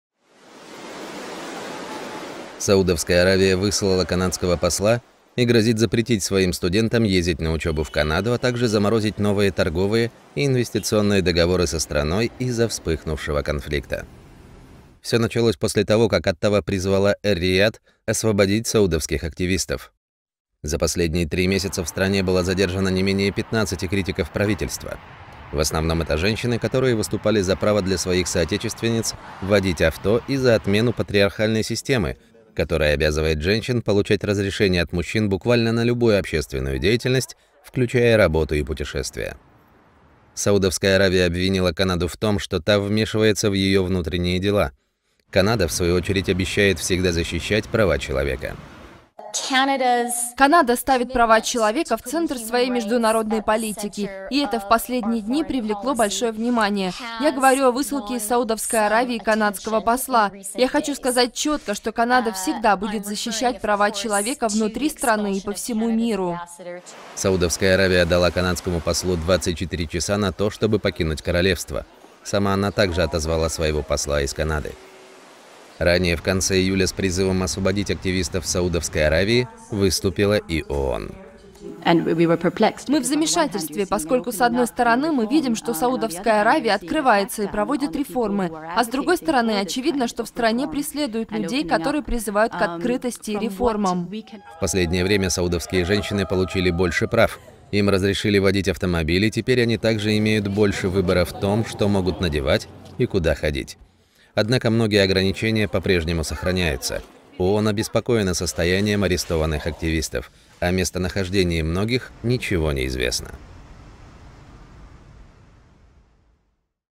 Саудовская Аравия выслала посла Канады и грозит разорвать с Оттавой торговые связи
После призыва Канады освободить правозащитных активистов Саудовская Аравия выслала канадского посла и грозит разорвать с Оттавой торговые и инвестиционные отношения. Канада, в свою очередь, заявляет, что всегда будет поддерживать права человека.